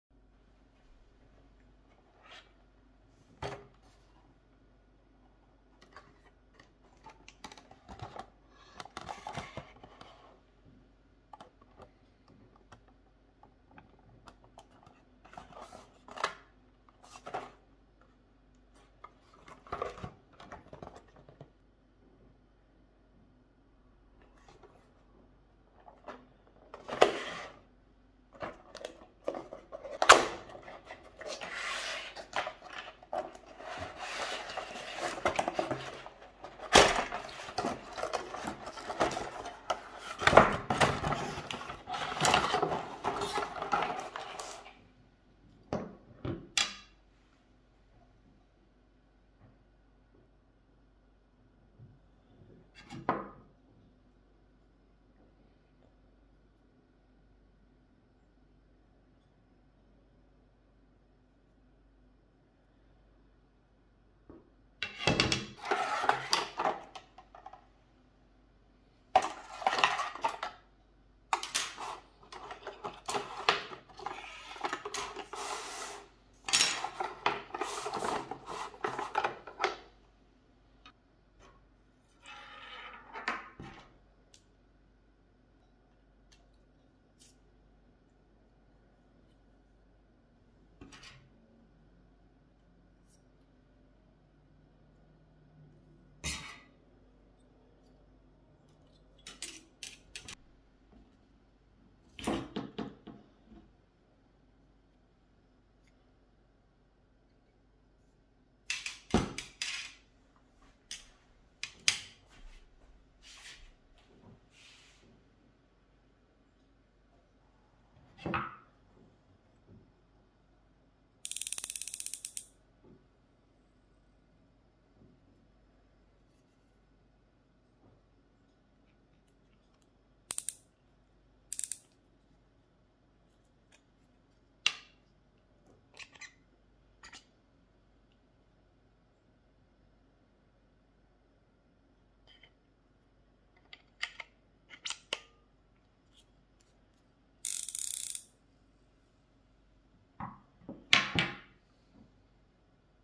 Unboxing Decathlon Caperlan fly fishing combo (rod, reel, line, leader) HRK 8'6 #4/5
Combo rod (8.5 ft / 2.59 m), reel, fly line (DT5F) and leader for fly fishing with dry flies and nymphs for trout and cyprinids over short and medium distances.